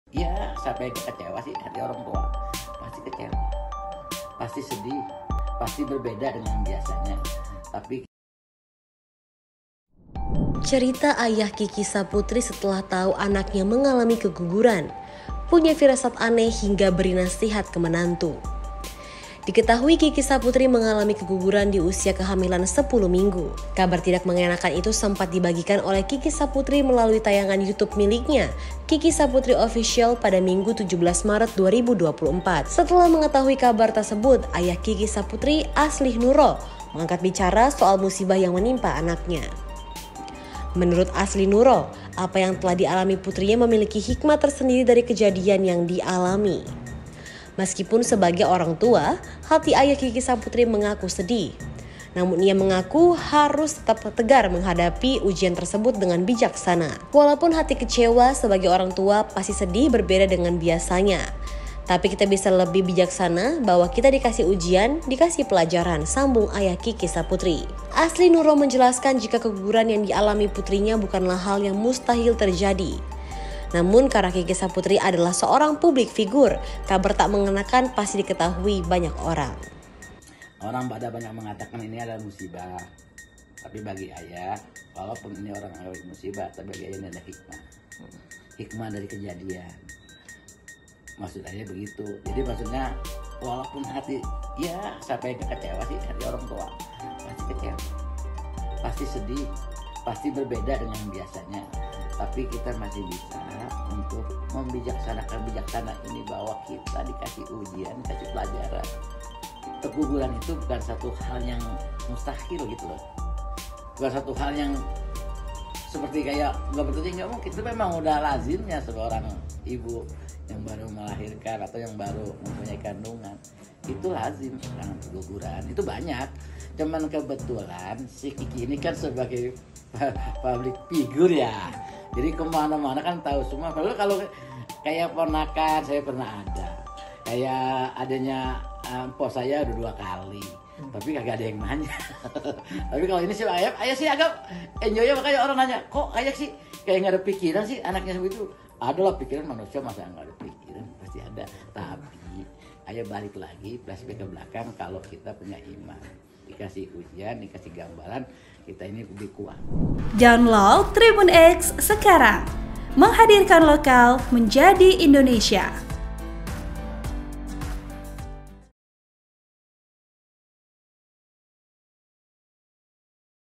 Ayah Kiky Saputri saat Tahu Anaknya Alami Keguguran, hingga Beri Nasihat ke Menantu
Cerita ayah Kiky Saputri setelah tau anaknya mengalami keguguran, punya firasat aneh hingga beri nasihat ke menantu.

Diketahui Kiky Saputri mengalami keguguran di usia kehamilan 10 minggu.

Kabar tidak mengenakan itu sempat dibagikan oleh Kiky Saputri melalui tayangan YouTube miliknya Kiky Saputri Official pada Minggu (17/3/2024).

Setelah mengetahui kabar tersebut, ayah Kiky Saputri, Aslih Nuro pun angkat bicara soal musibah yang menimpa anaknya.

MUD